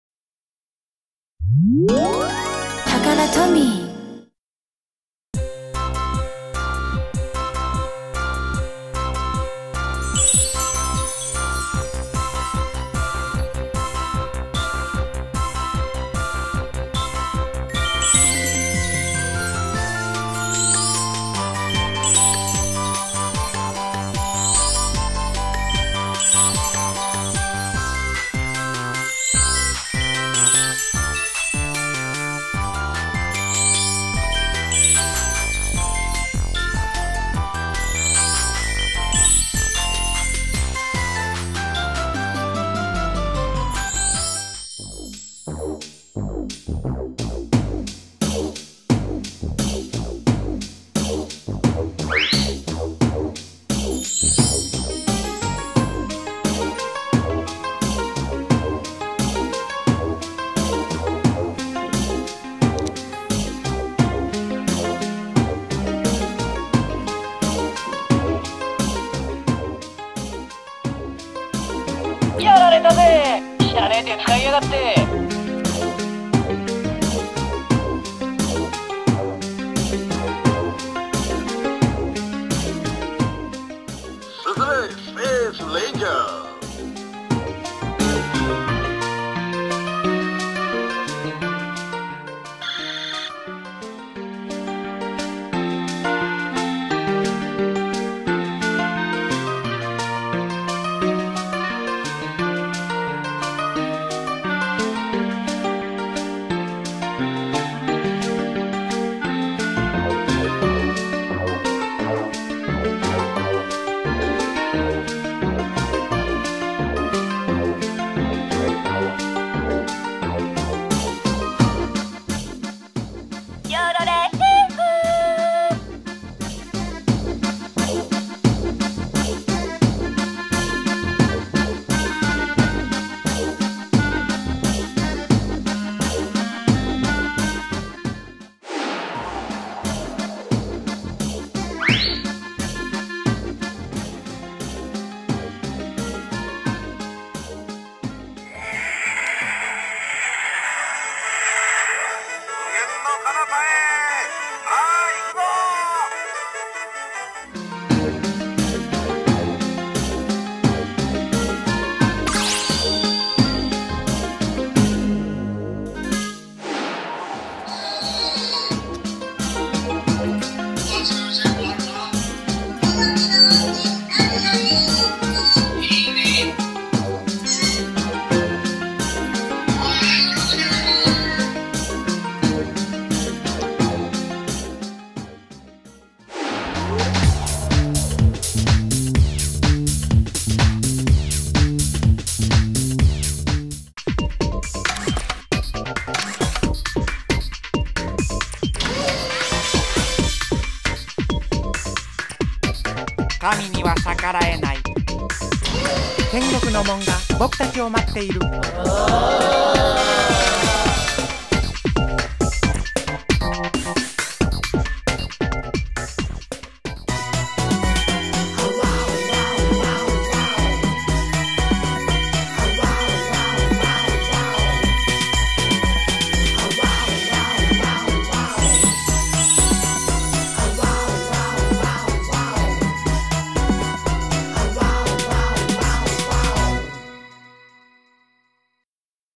タカラトミーのトイ・ストーリーのおもちゃたちと写真を撮ってみんなでＷＥＢＣＭをつくろう！
. ２０１４年夏に募集しました「タカラトミーのトイ・ストーリーのおもちゃたちと写真を撮ってみんなでＷＥＢＣＭをつくろう！キャンペーン」のＷＥＢＣＭが完成したよ！
たくさんのご応募ありがとうございました。ステキな写真がいっぱいでスタッフも感激！！キミの写真はあるかな？